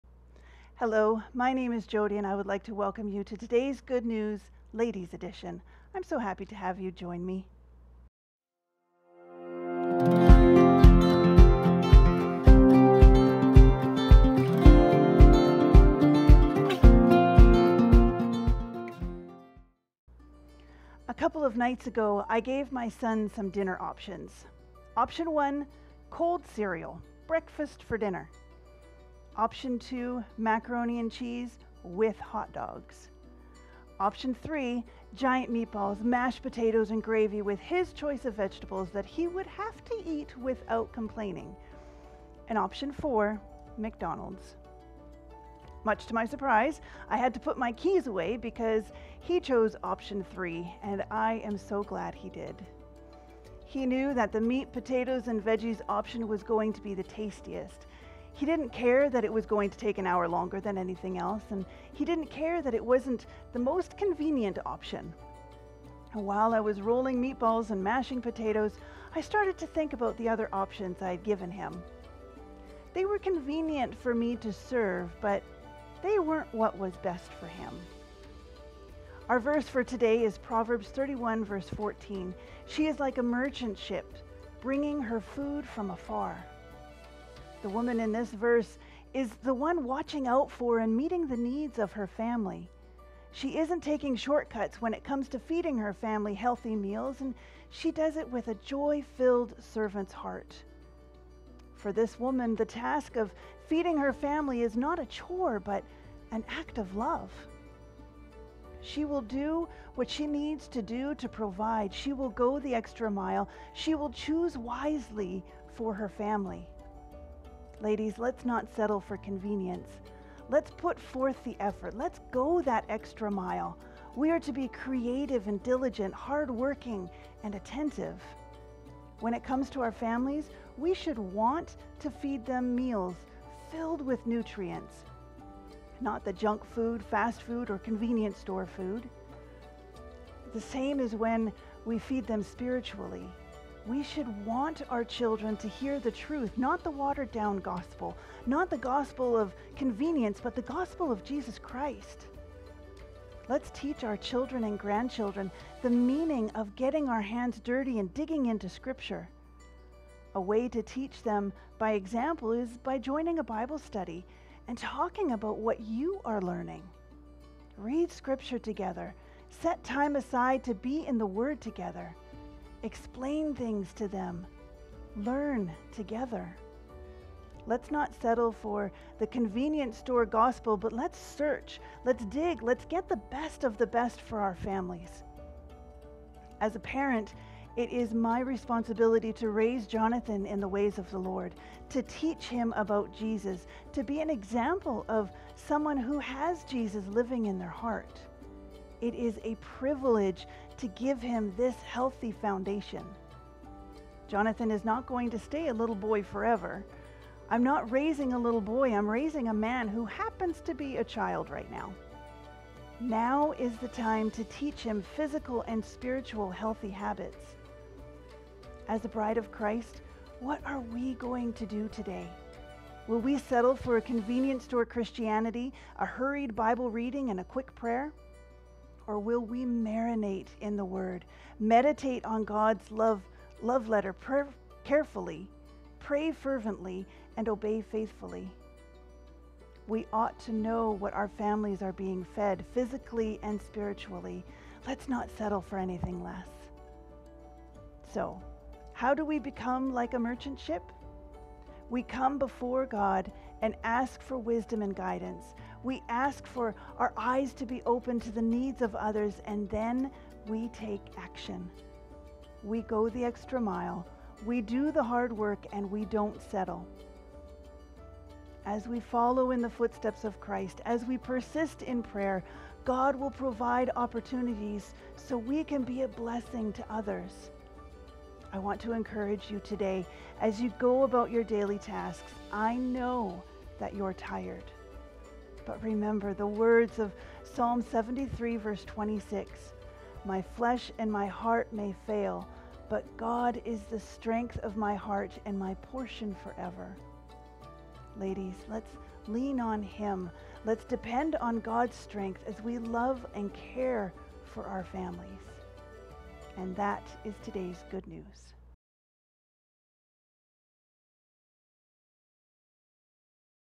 Proverbs 31:14
Join us for our devotional series “today’s good news” Ladies Edition in the book of Proverbs with Jody Gonzales